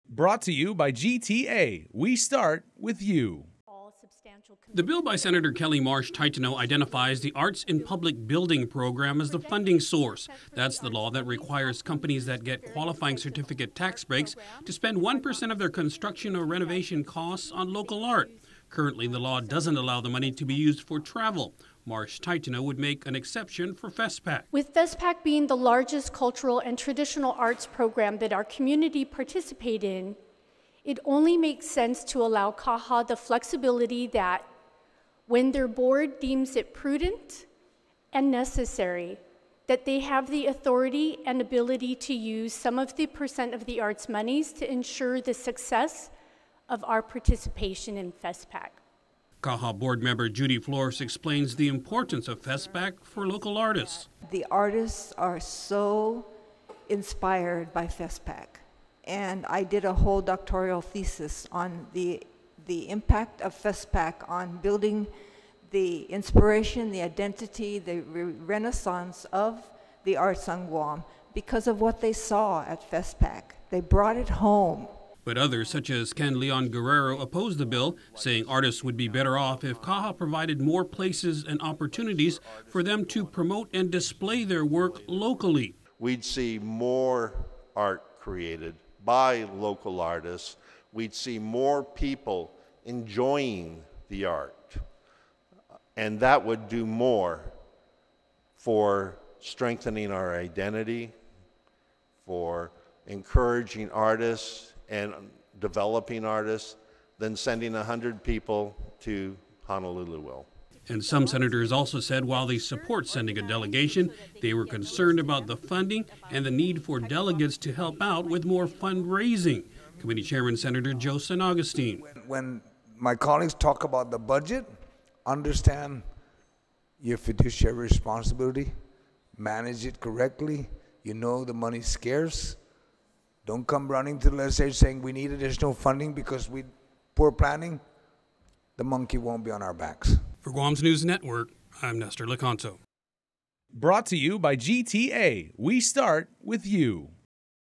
Marsh (Taitano) suggests funding source for FestPac
Senators in session today discussed a bill to establish a dedicated funding source for Guam's participation in the Festival of the Pacific Arts. Officials want to spend up to $400,000 to send 100 artists to Honolulu next year. But critics say the money would be better spent helping artists promote their work here at home.